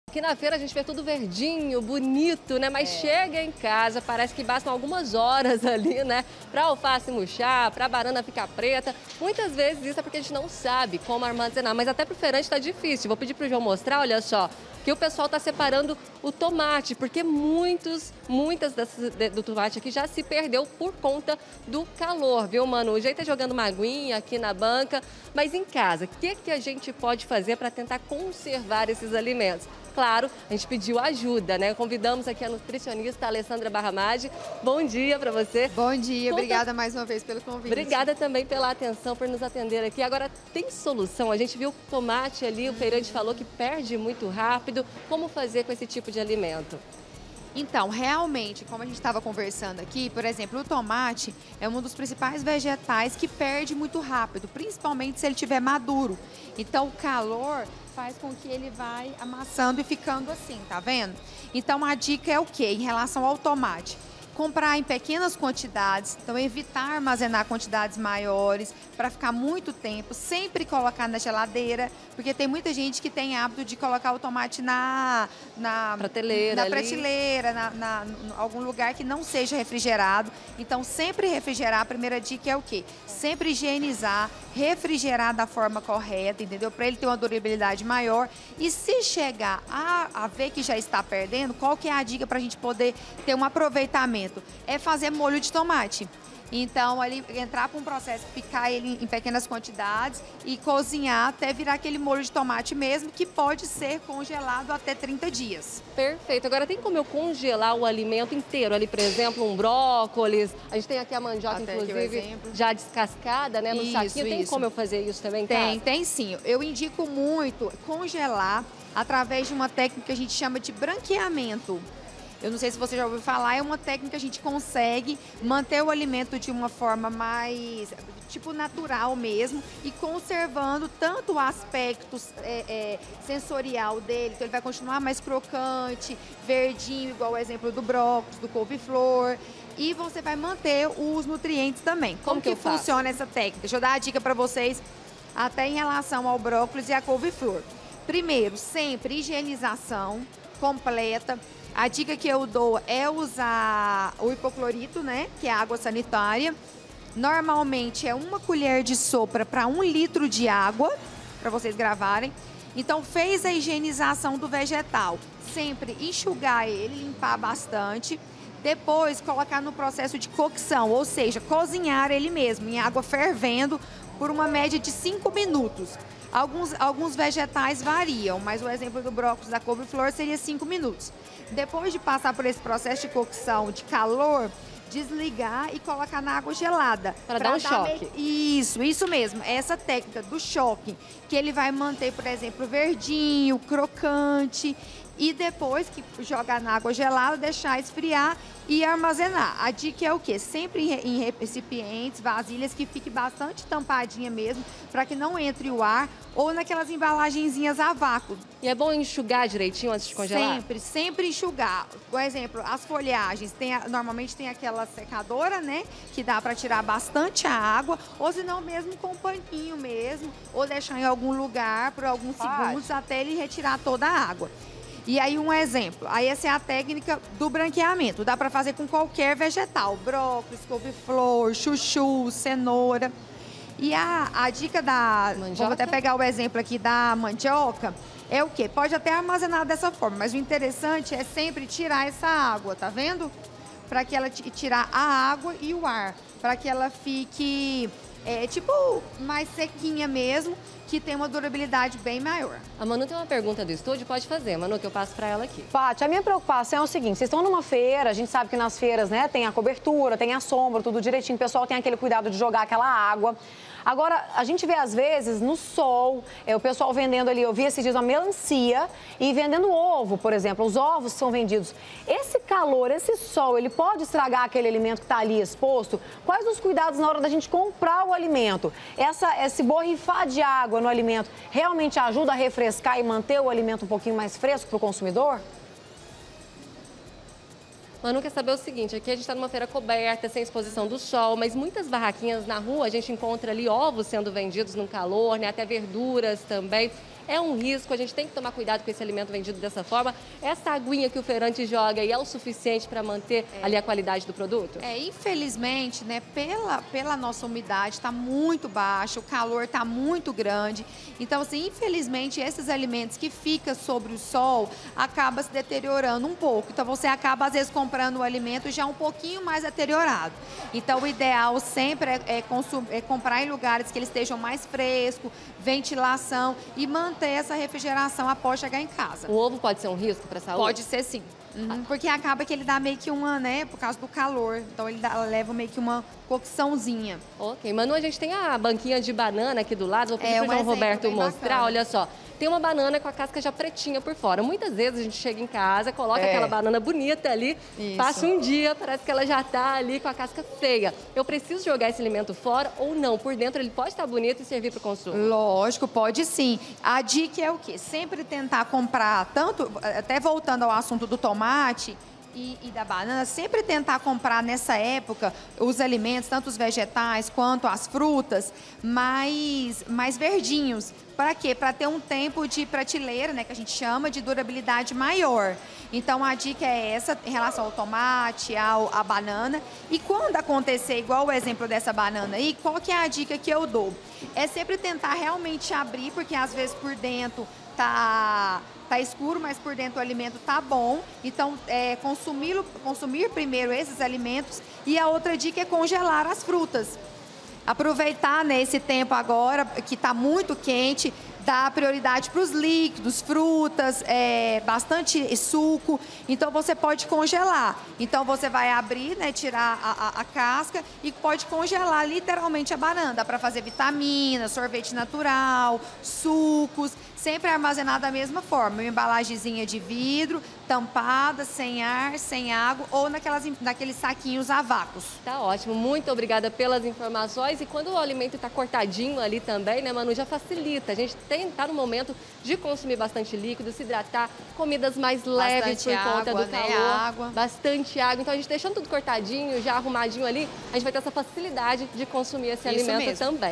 TEMPERATURAS ALTAS: COMO CONSERVAR BEM OS ALIMENTOS?
E POR CAUSA DESSE CALOR, MUITOS ALIMENTOS ACABAM APODRECENDO COM MAIS FACILIDADE AS FRUTAS E VERDURAS NÃO GOSTAM MUITO DO CALOR/ AQUELE ABRE E FECHA DE GELADEIRA NÃO DEIXA CONSERVAR BEM OS ALIMENTOS. PATRÍCIA PIASSA, COMO VOCÊ FAZ PARA MANTER OS ALIMENTOS POR MAIS DIAS?